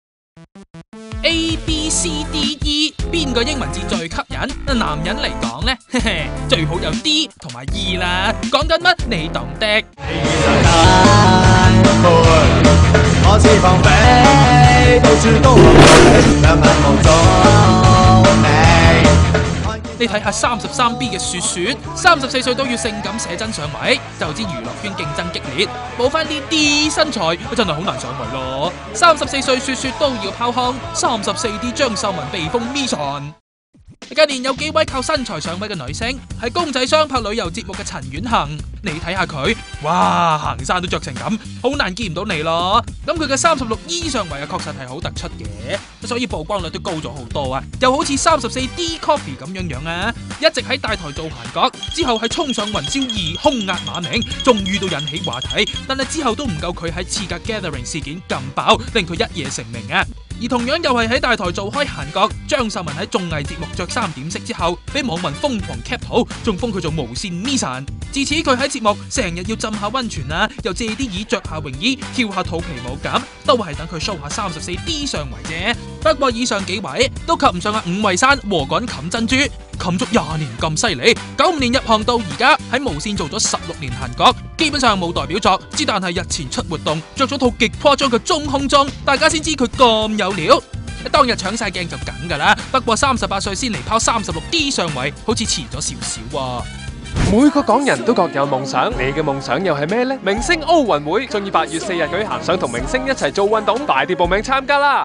34歲雪雪都要拋胸 34D張秀文被封咪神
娛樂圈競爭激烈，女星要上位非出法寶不行，擁豐滿上圍的當然較有利，近年有不少女星靠豐滿上圍成功上位。